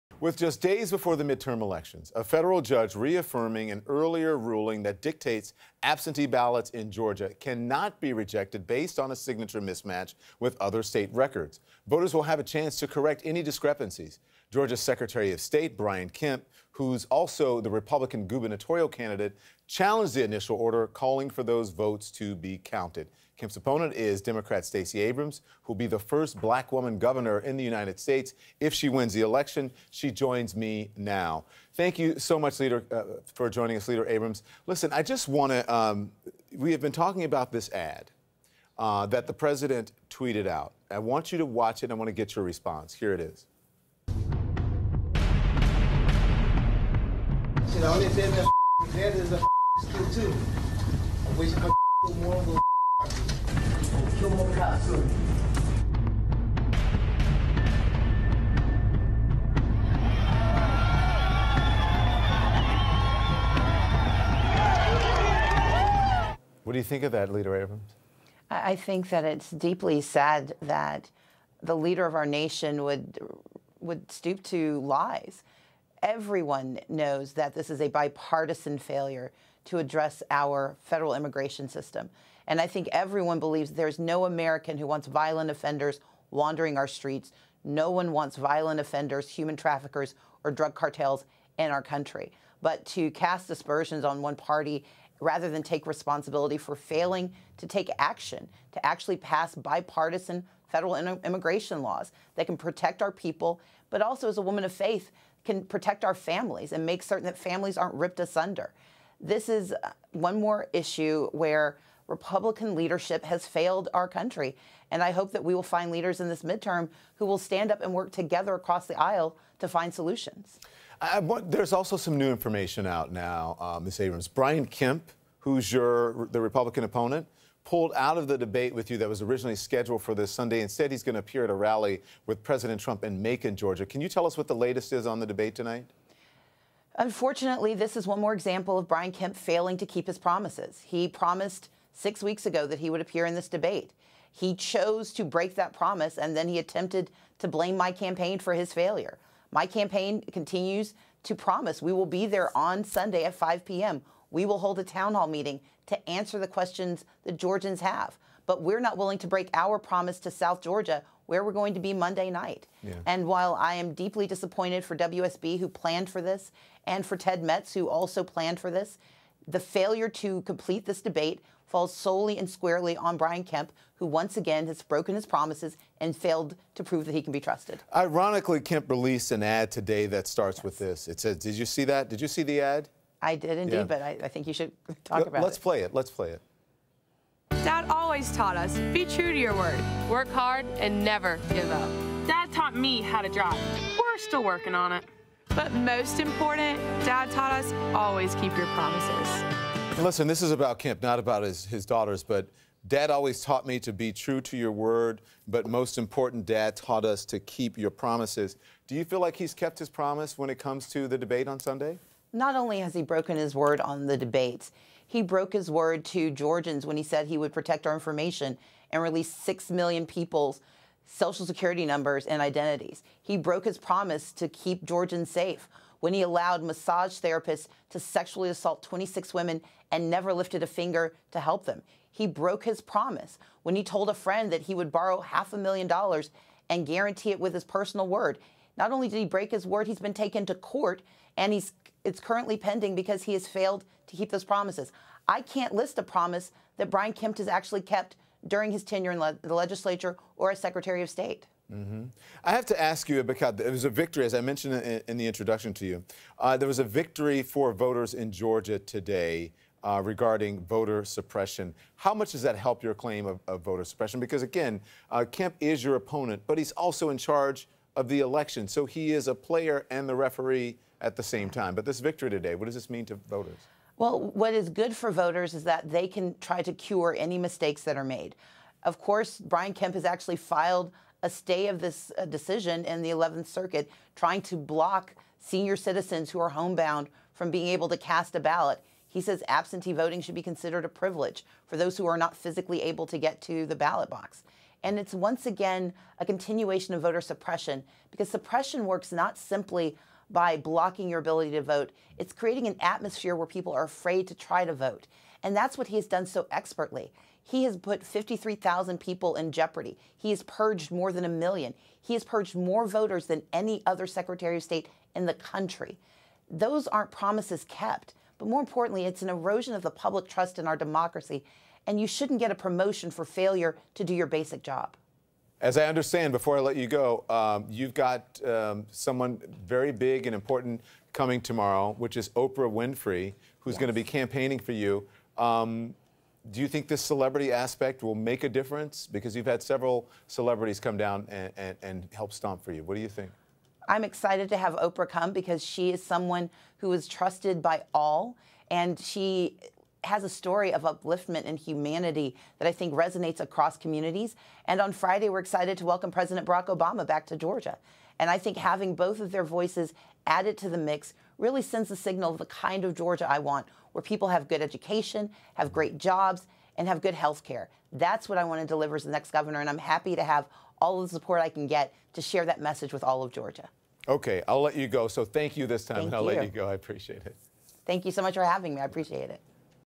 Stacey Abrams on Kemp He broke his promise to Georgians CNN
Georgia gubernatorial candidate Stacey Abrams sits down with CNN’s Don Lemon to discuss her political campaign a week ahead of the 2018 midterm elections.